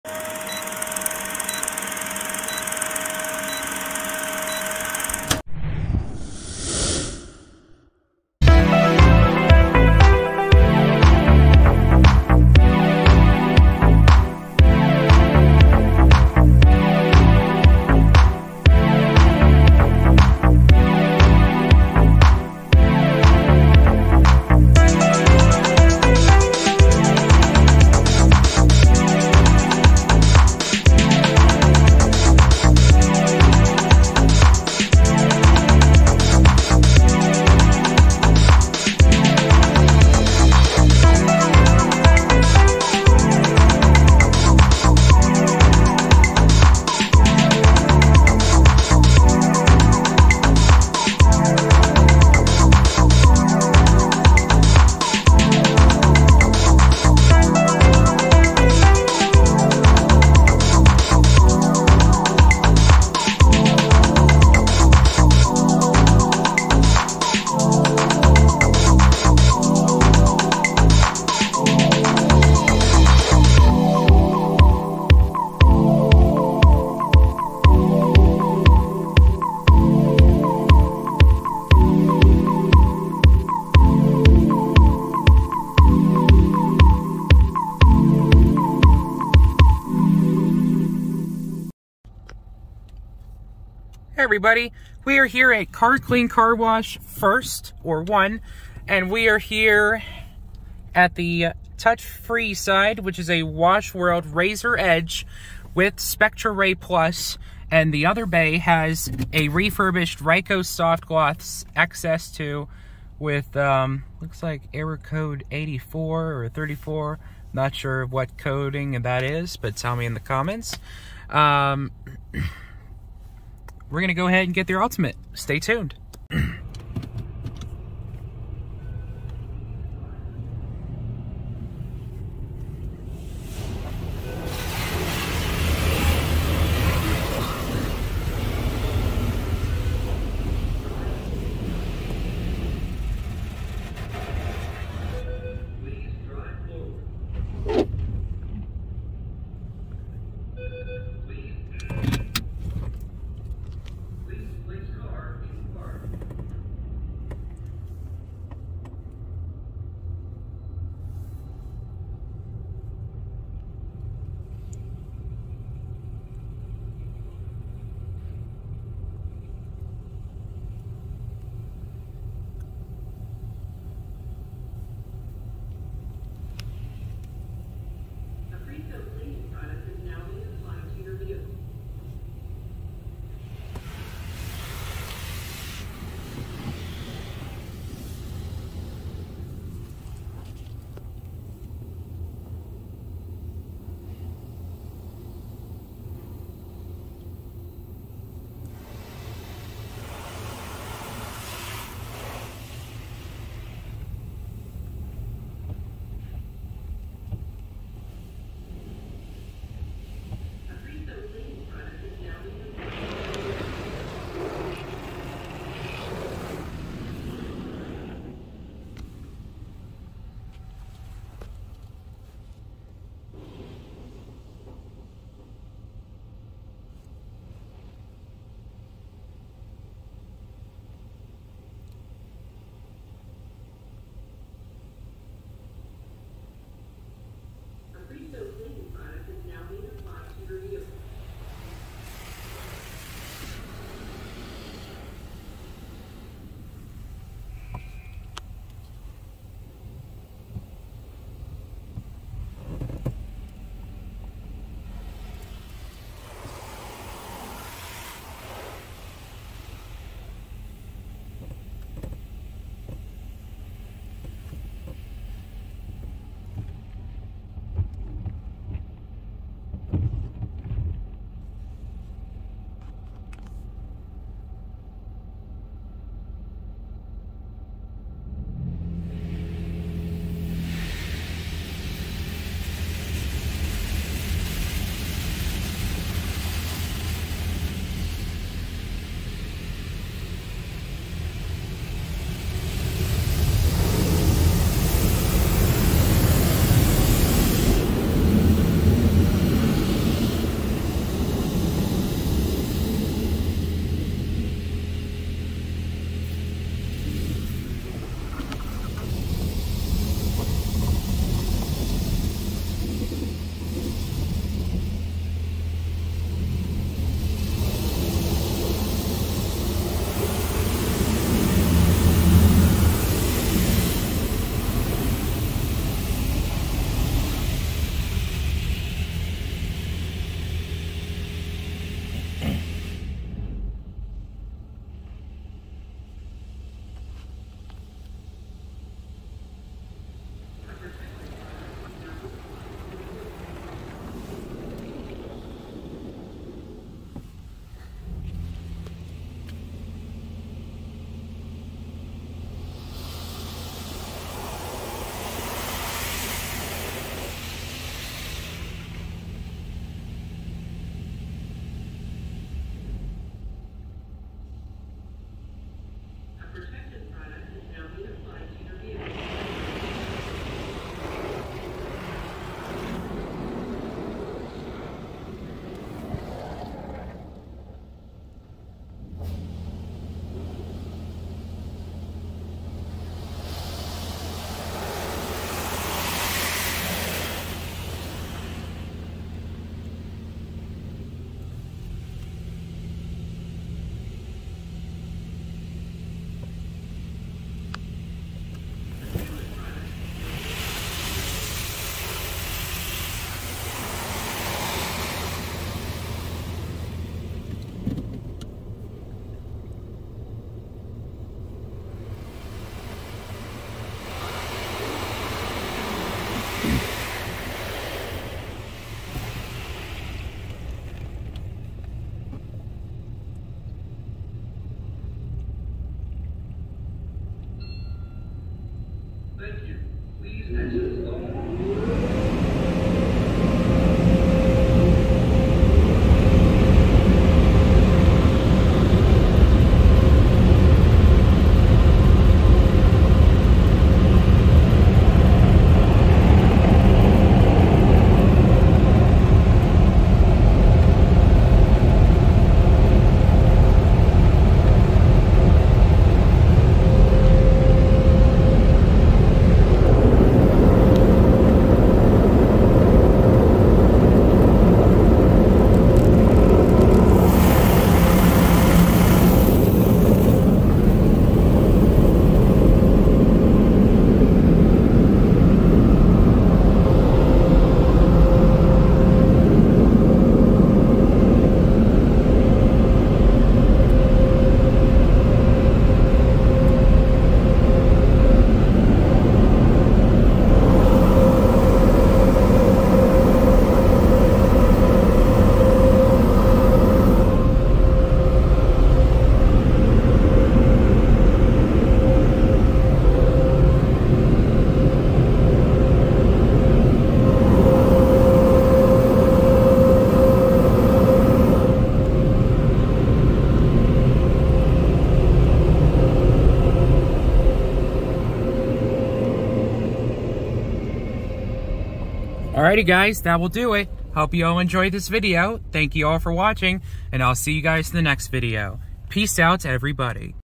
Washworld Razor with Spectraray+
Here is an amazing Washworld Razor with SpectraRay+ on the sides. The dryer section has two older dryers, which makes me think there probably was an older system before. This wash is very nice, and gave me much dryer time!